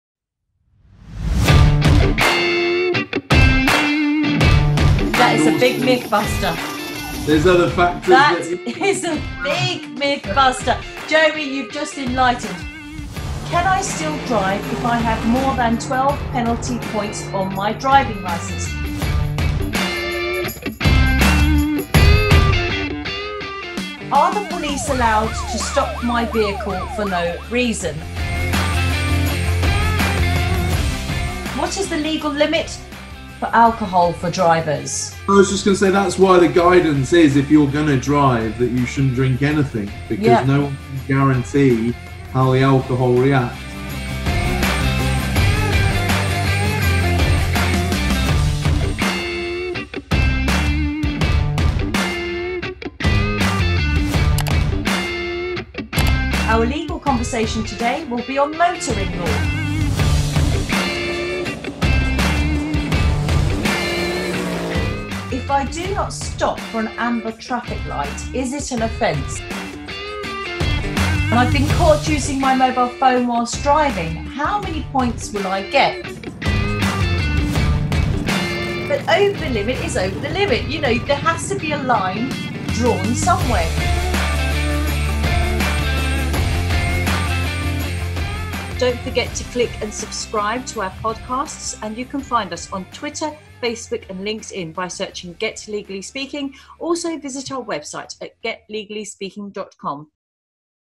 Preview of our Motoring Law Podcast - Myth Buster!!
Join us in this myth busting conversation on Motoring Law, in which Hatti Suvari and barrister Jeremy Rosenberg discuss whether:
-  12 penalty points would mean that your driving licence is taken away
-  the rights that the Police have to stop your vehicle
-  what is meant by the drink driving 'legal limit', and
-  the sentencing guidelines for drink driving offences.

A great Podcast giving bags of free legal conversation on various aspects of motoring law, including the penalties linked to holding or using your mobile phone whilst driving, and whether driving through an amber traffic light is a motoring offence. A not to be missed episode for drivers!